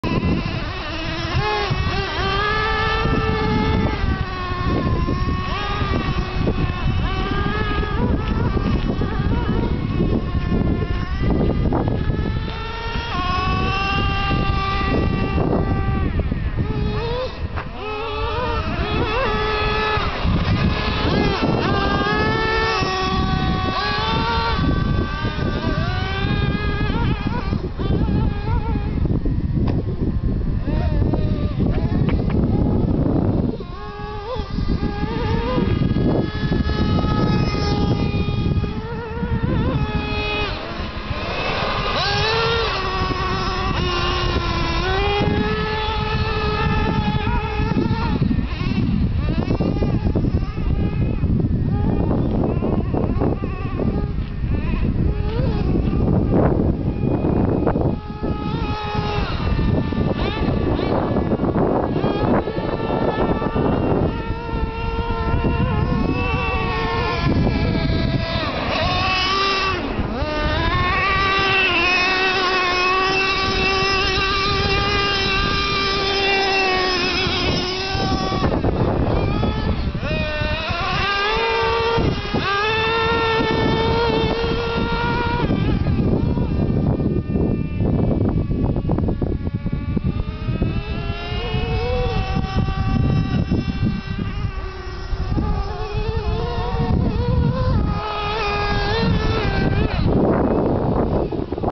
Revo 2.5r and Jato 3.3
Having a blast around a hockey pitch and on grass. Crash at 30secs!!! no damage thank god!